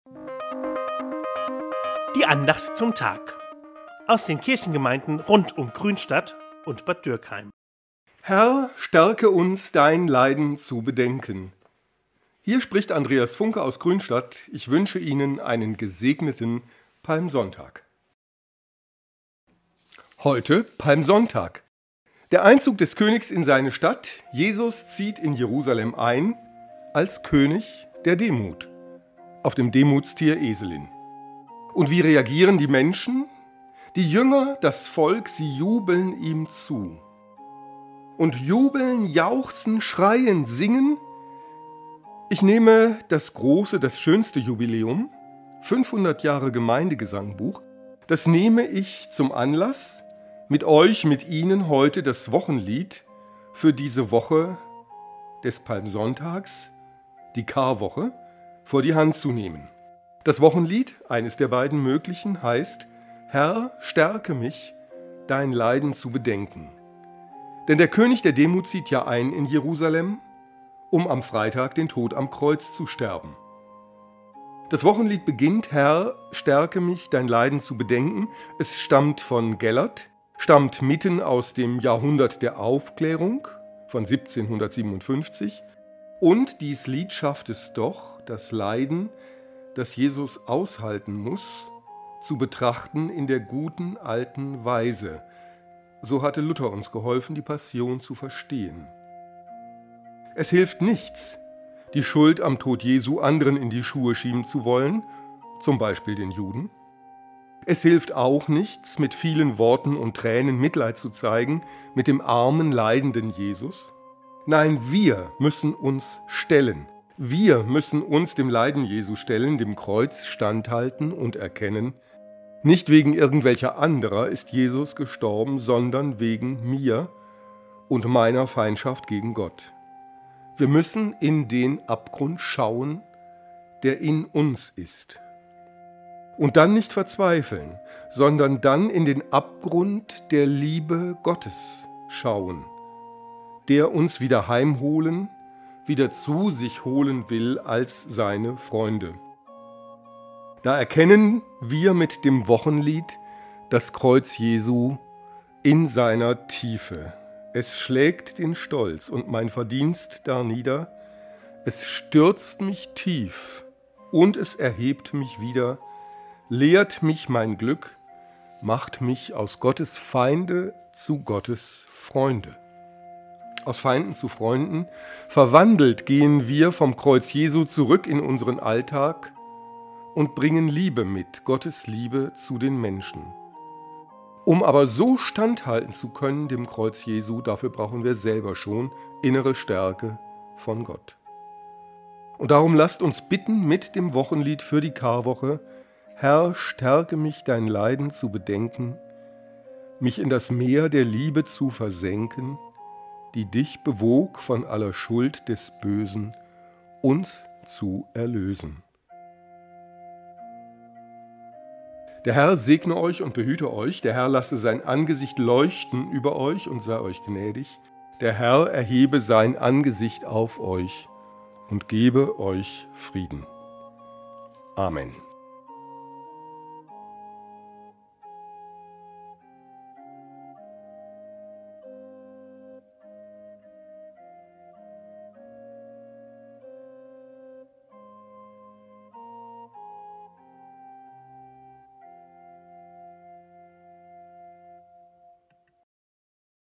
Podcast #1473 aus dem Leininger Land am 24.März 2024 mit Andreas Funke: Leiden bedenken
Heute: Podcast #1473 aus dem Leininger Land am 24.März 2024 mit Andreas Funke, Pfarrer in Grünstadt: Leiden bedenken